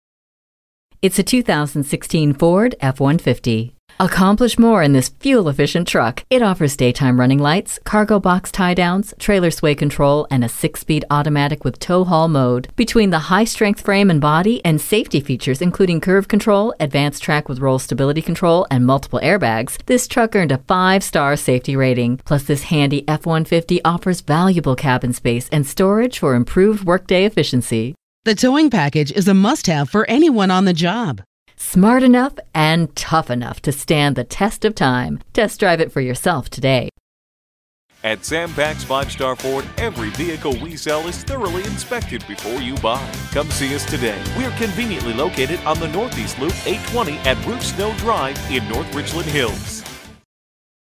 New 2016 Ford F-150 North Richland Hills, TX
Year: 2016
Make: Ford
Model: F-150
Trim: Lariat
Engine: EcoBoost 3.5L V6 GTDi DOHC 24V Twin Turbocharged
Transmission: 6-Speed Automatic Electronic
Color: Gray
Stock #: GFC67492
VIN: 1FTEW1EGXGFC67492

Address:
6618 NE Loop 820